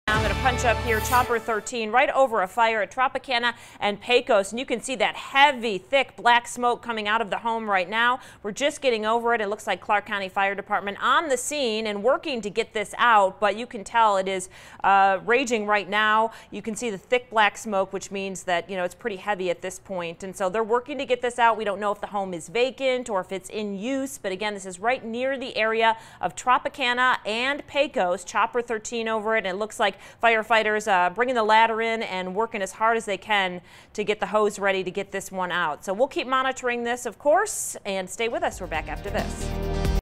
House on fire in the area of Tropicana Avenue and Sandhill Road
Firefighters  working to put out a house fire near Tropicana and Sandhill.